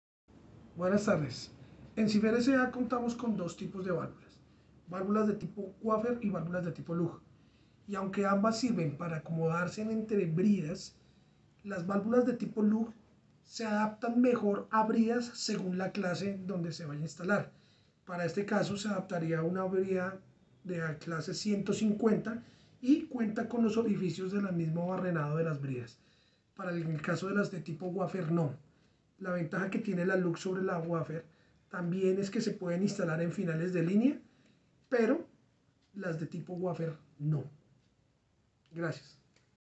Válvula tipo wafer o lug ¿cuál comprar?
En Sifer SA ofrecemos válvulas mariposa tipo wafer o tipo lug y aunque las dos sirven para acomodarse entre bridas no son iguales.